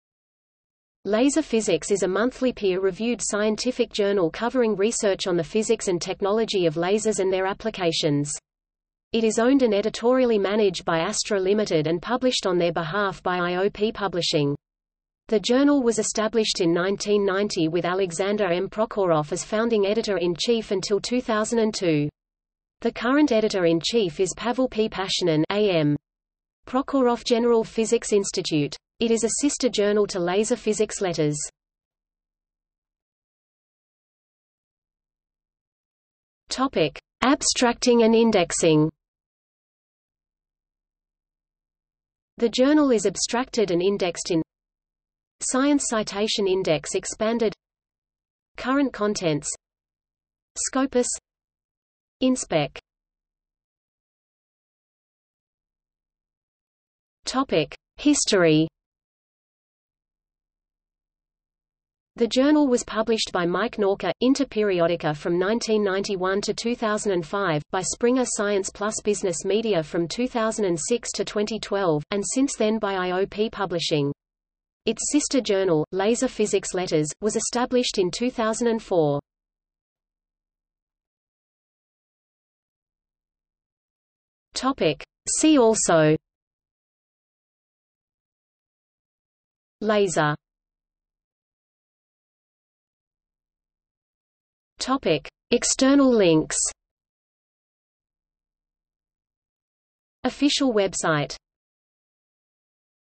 Laser Physics (journal) | Wikipedia audio article
00:00:33 1 Abstracting and indexing
00:00:53 2 History
00:01:21 3 See also
00:01:31 4 External links

- Socrates

SUMMARY
Laser Physics is a monthly peer-reviewed scientific journal covering research on the physics and technology of lasers and their applications. It is owned and editorially managed by Astro Ltd. and published on their behalf by IOP Publishing. The journal was established in 1990 with Alexander M. Prokhorov as founding editor-in-chief until 2002. The current editor-in-chief is Pavel P. Pashinin (A.M. Prokhorov General Physics Institute). It is a sister journal to Laser Physics Letters.